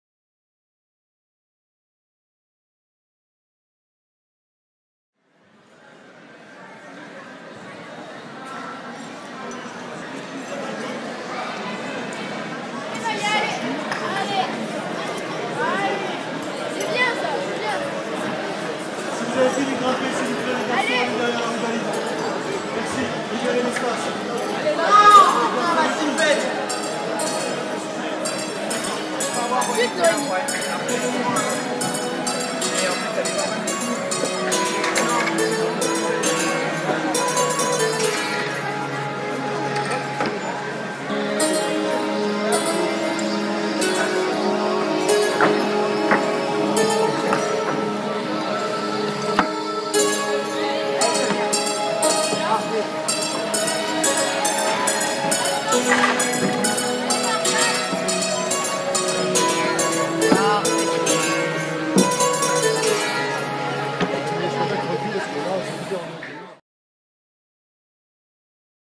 Digne capitale académique de l'escalade
Digne-les-Bains accueille ce mercredi 4 avril les Championnats d'académie d'escalade